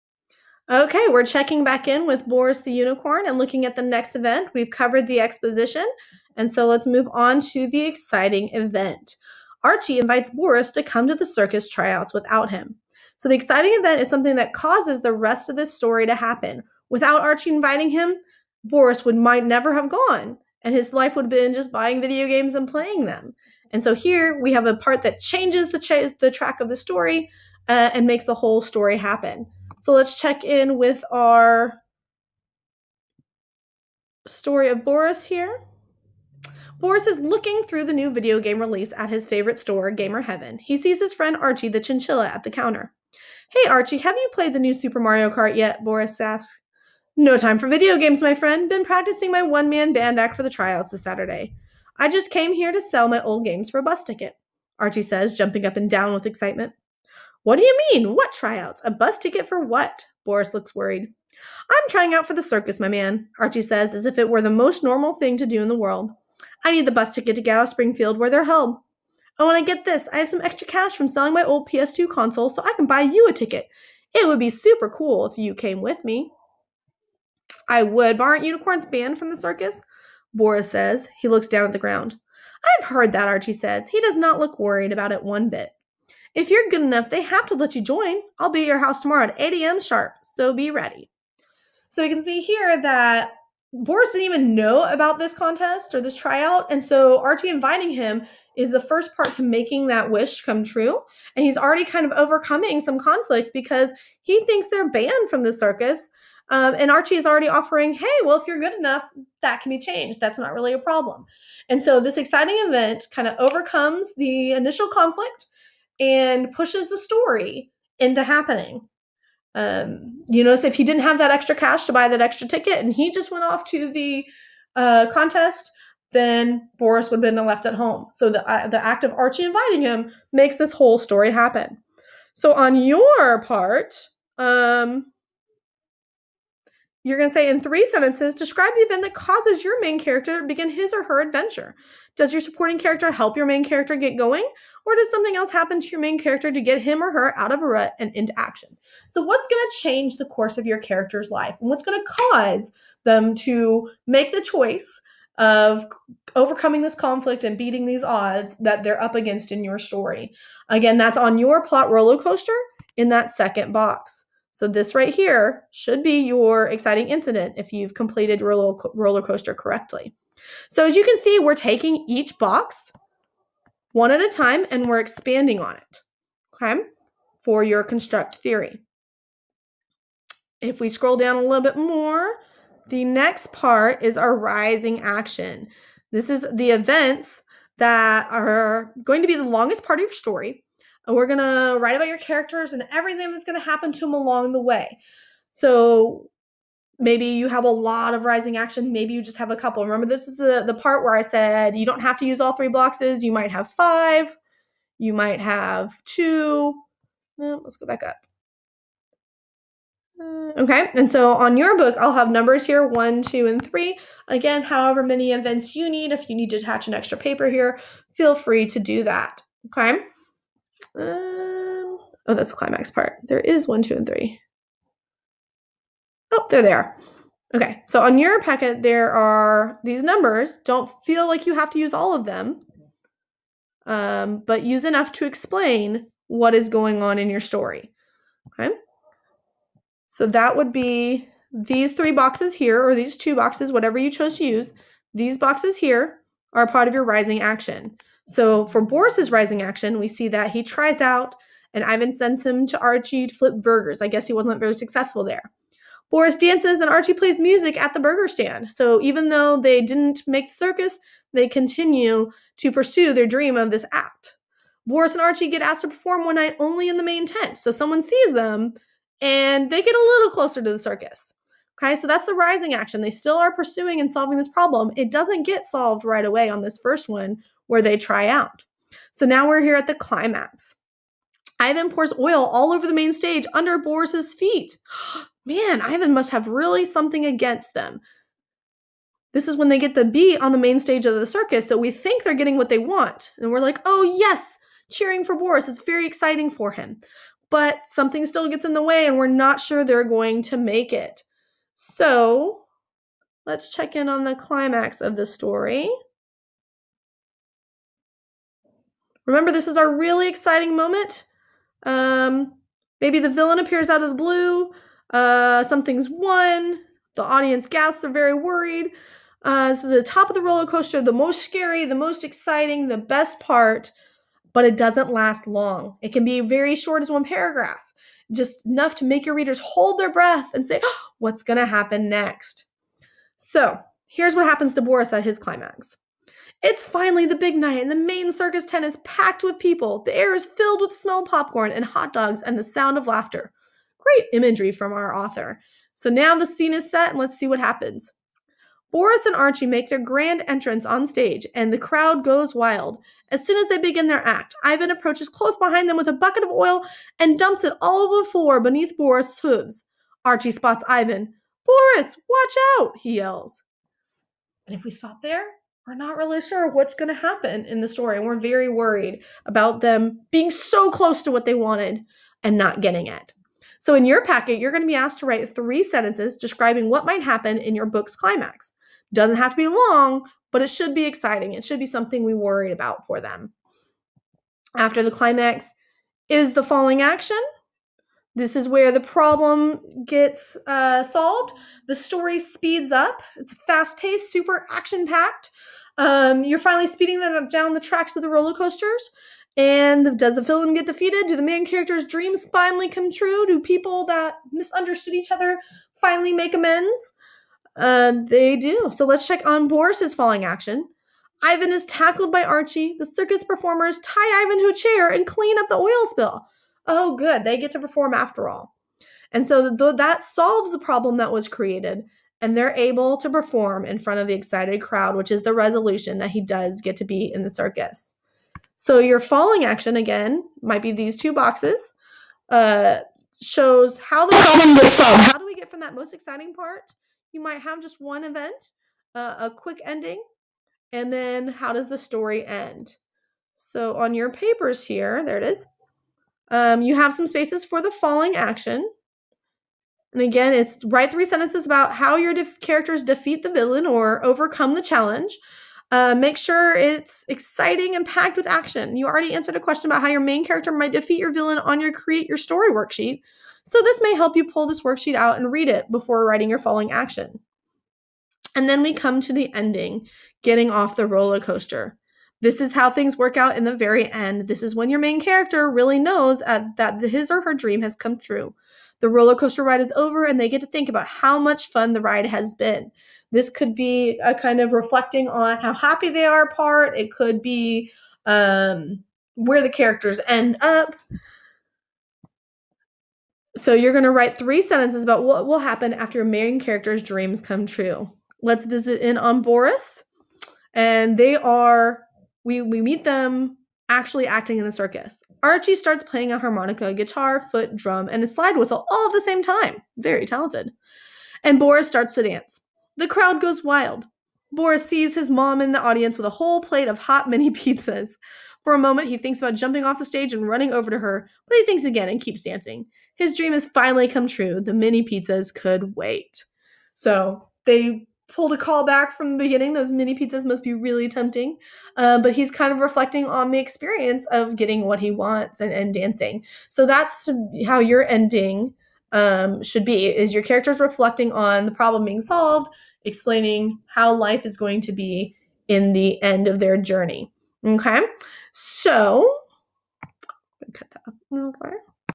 Construct Theory - Fiction Part 3
Explanation and examples of how to complete the rest of the construct theory packet - exciting incident through resolution.